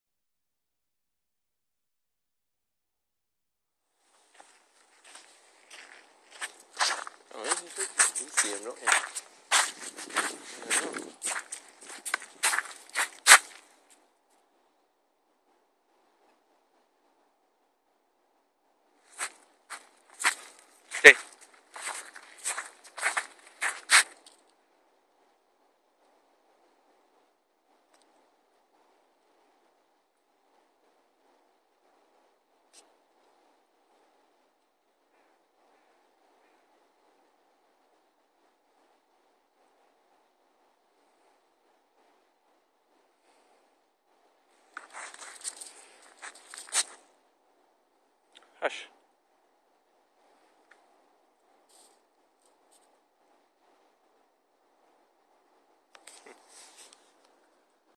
Coyote hunting
Coyote pouncing on rodents